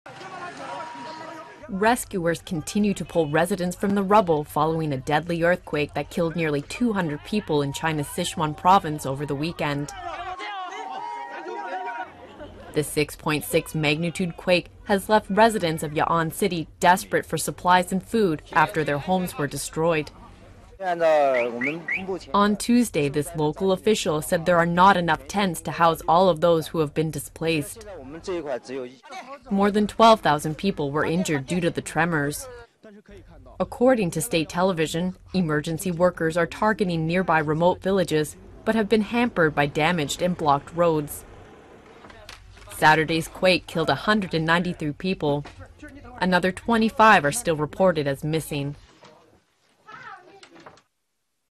Rescue efforts continue in quake-stricken Sichuan province
Chinese residents are still being pulled from rubble following a deadly quake that struck Sichuan province over the weekend, killing 193. Jessica Gray reports.